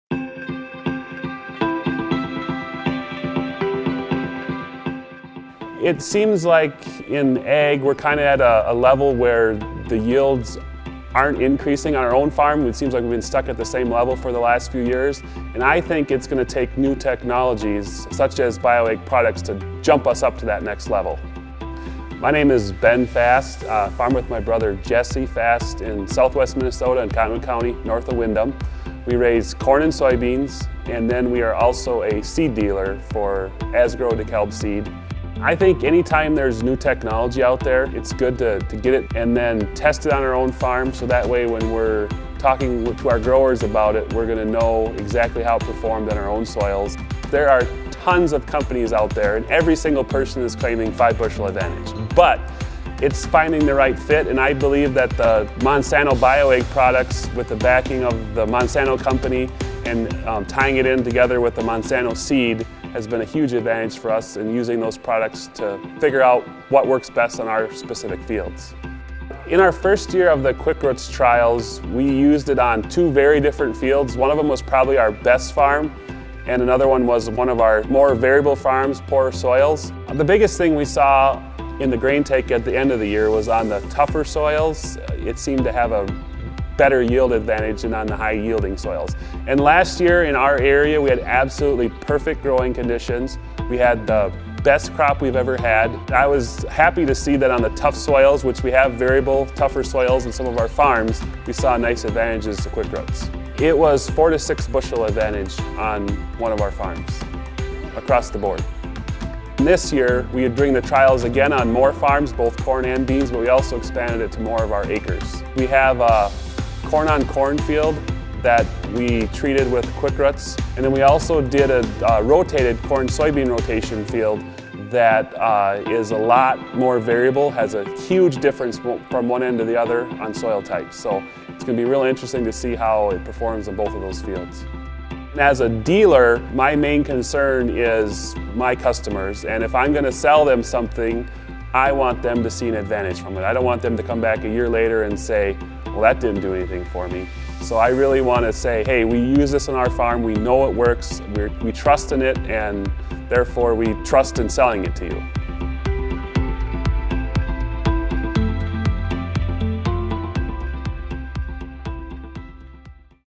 QuickRootsⓇ Technology: Yielding Better Results
Growers know that, in the right conditions, new technology can give them advantages in the field. That’s why Ben Fast tested QuickRootsⓇ Technology from Monsanto BioAg™. After seeing improved yields on two very different fields, he’s sold on the product’s performance.

Experience more trial participant stories at MonsantoBioAg.com/BATrials